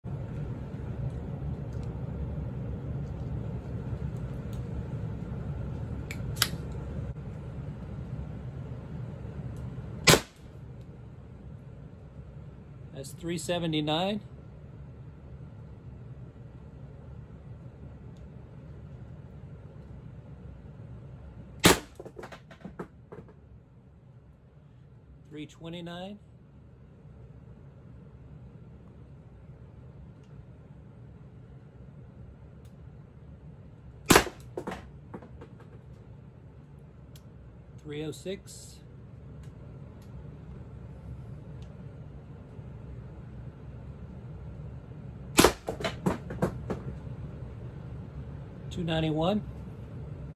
Pepperball TCP, 53 Joules cheap and easy!
Powerful and deadly accurate with Grimburg JULv2. Here are the first 4 shots using a cheap 9" smoothbore eBay barrel (0.686 bore) and Grimburg JULv2 in my PepperBall TCP.  I still think about whether this is a reasonable size to carry around but typically just run the 5" Grimburg barrel. The power jump of more than 20 Joules from the 5" barrel is truly impressive. This also shows that you don't need anything fancy to get great power from this platform. You don't need expensive barrels or upgrades to increase the air chamber capacity. This is more than enough power for less lethal defense applications. Most importantly, running this amount of air through the bolt is safer and will be better for long-term durability. At the end of the day these need to be reliable. Period.

To watch the full-length video please follow this link and enjoy.